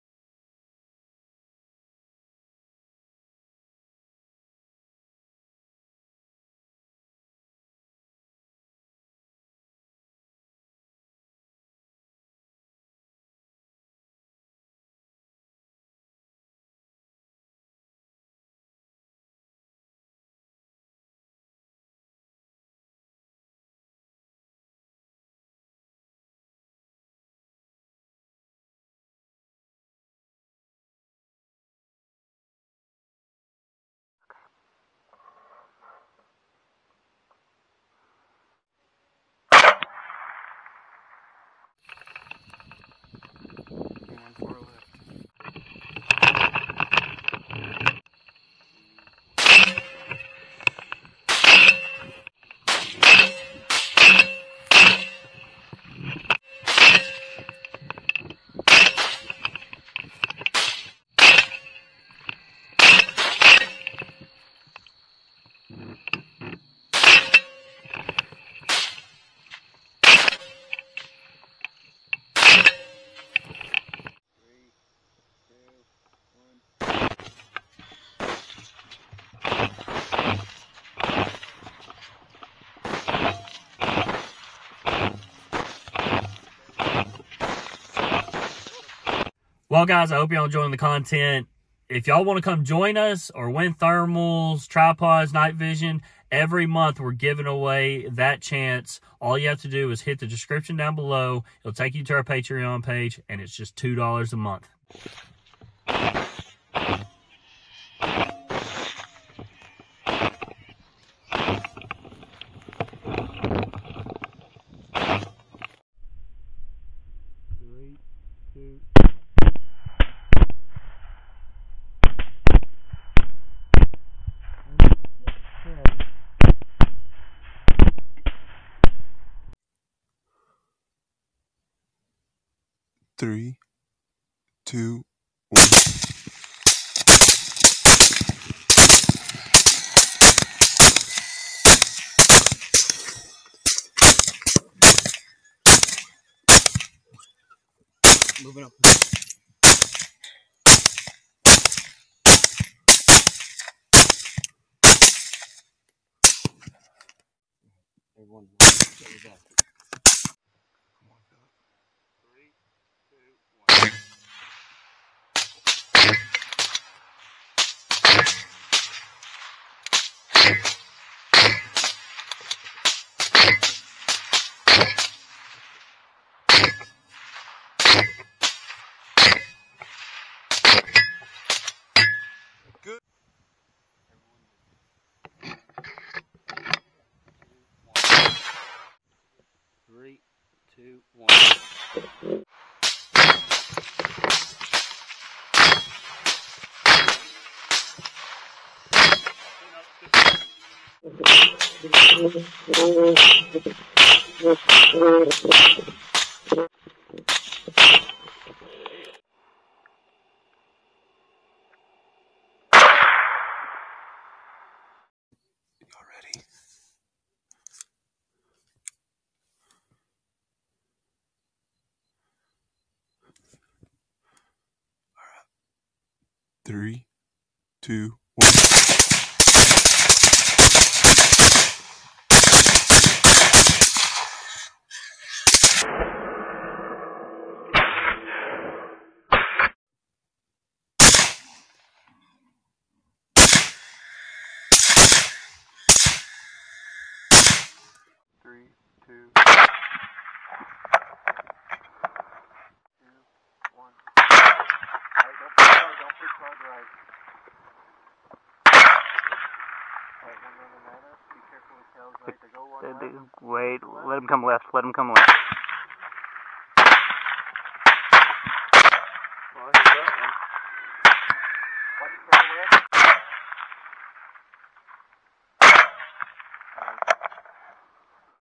41 WILD HOGS at the Dairy
This weeks recap: The Feral Hog problem in Texas is far from over but we are making the best of it.

Thermals used: Pulsar Thermion XL50 LRF
Pulsar XP50 Trail 2
Pulsar Thermion XP50 LRF
Pulsar Mergers XL50 LRF